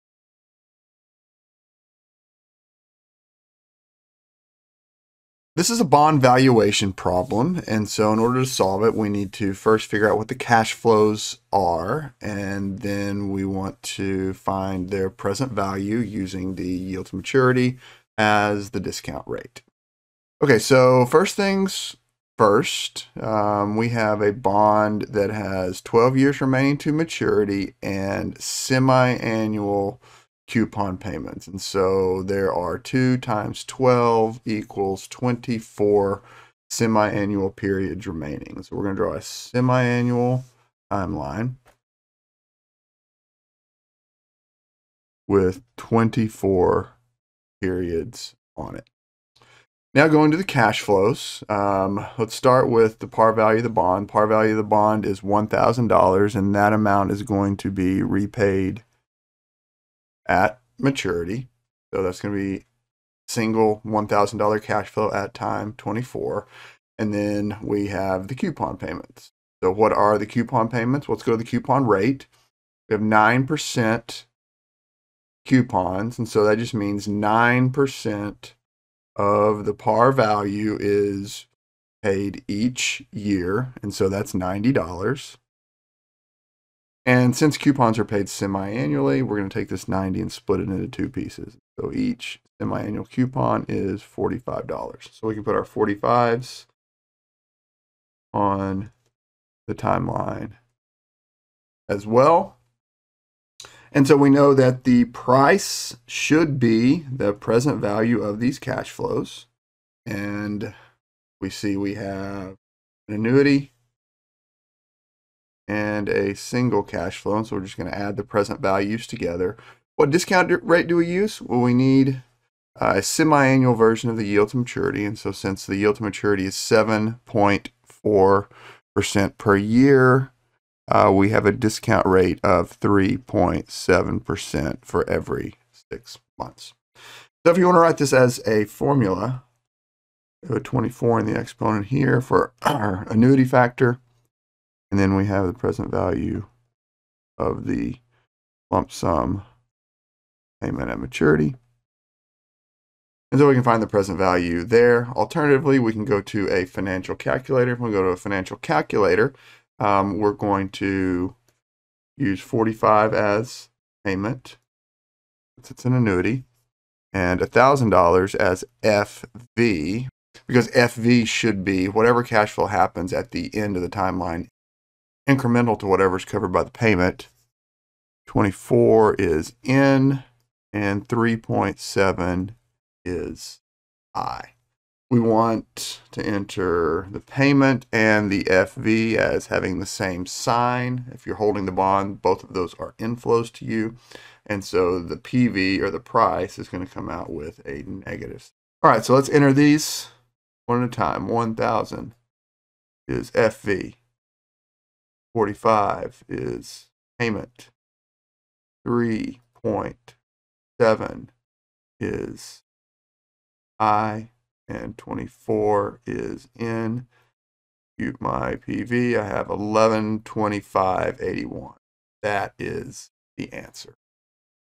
How to compute a bond price
I compute the price of a coupon bond using a financial calculator.